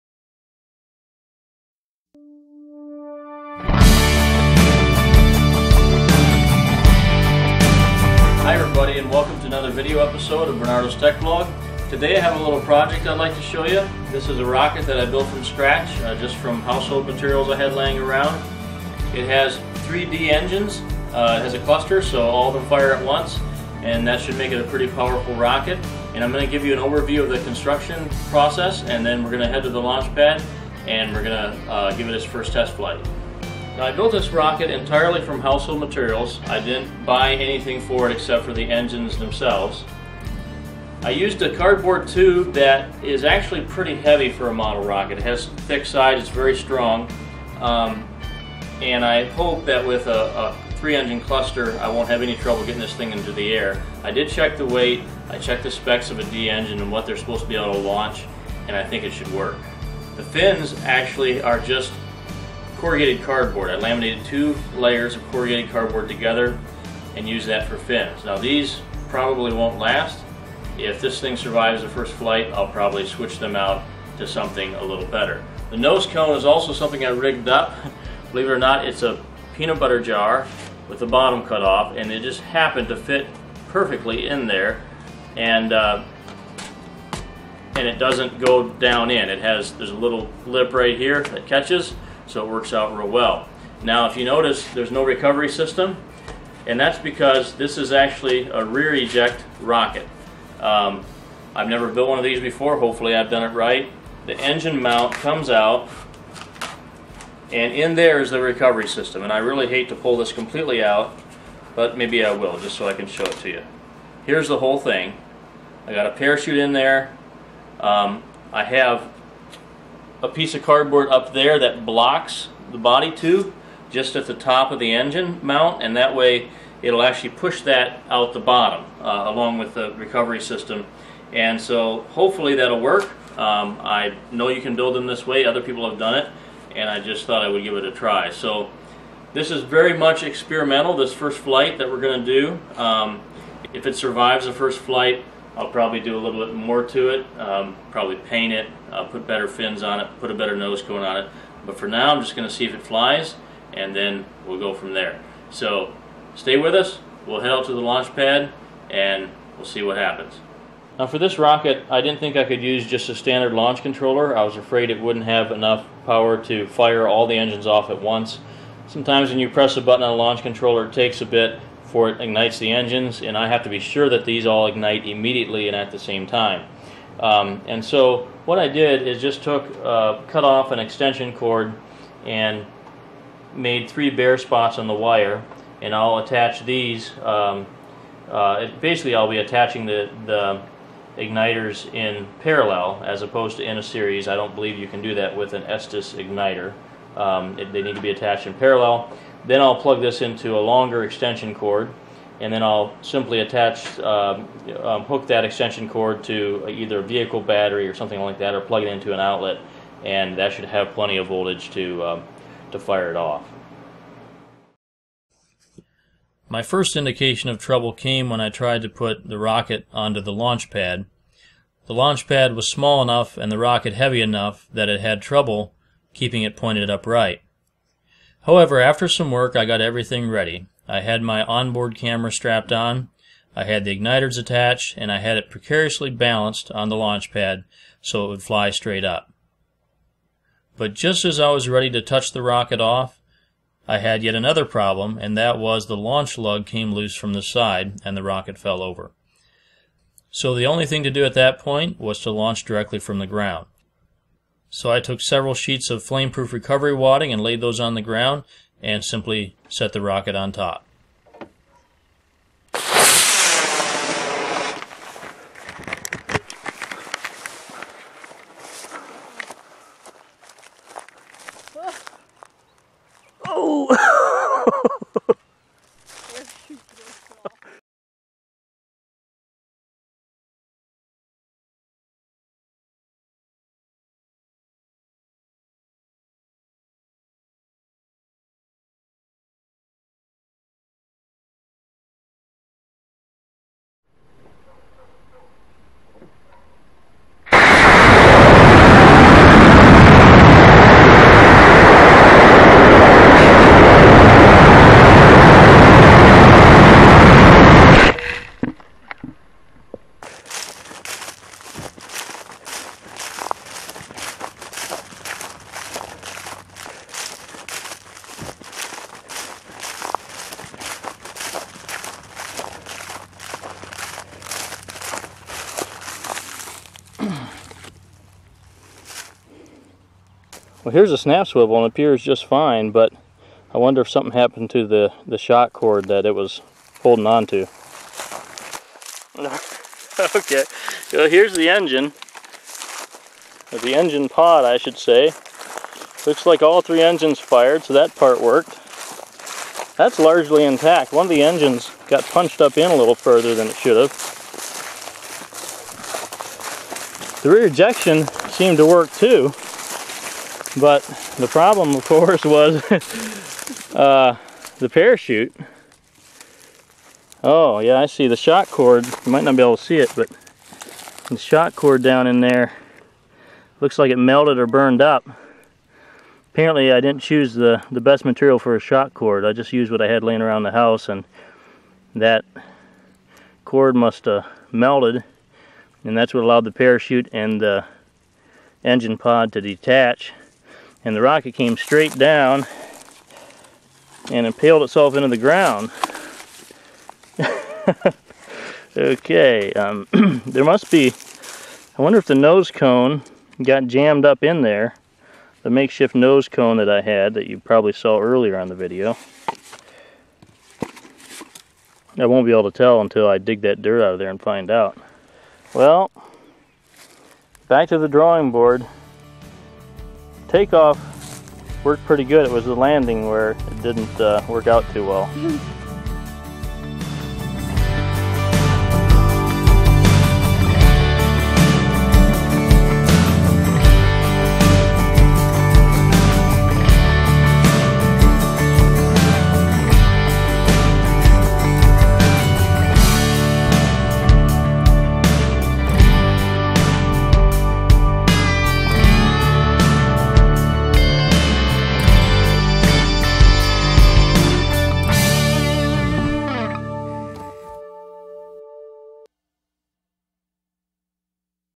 Homemade rocket w/ D engine cluster - build, launch, and crash
Building and launching my built-from-scratch, homemade model rocket.  This rocket was powered by a cluster of three size D engines from Estes.  In-flight footage taken by an 808 #16 key-chain camera.  The rocket crashed landed because the shock cord broke, but it will be repaired.